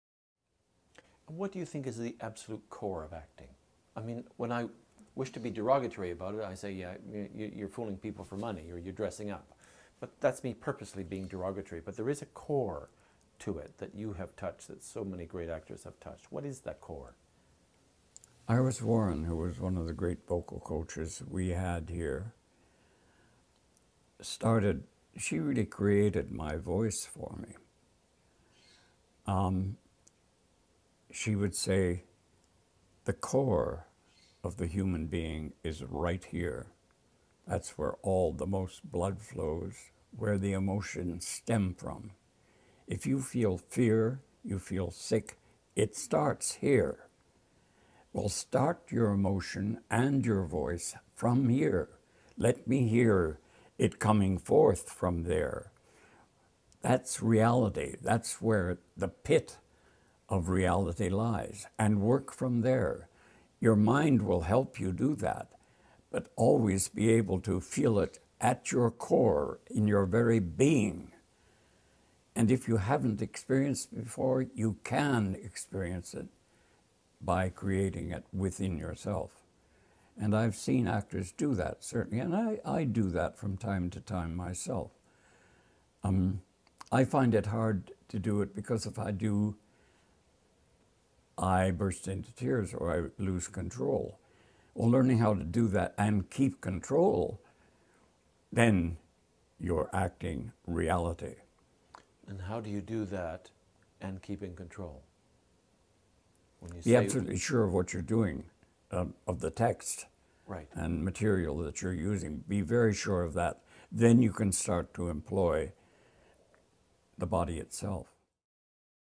William Needles on the core of acting (Part 1 of 25)
William Needles on the source of voice and emotion, and how both can be used with the text.

Interviewed by R.H. Thomson on September 27, 2005 in Stratford, ON.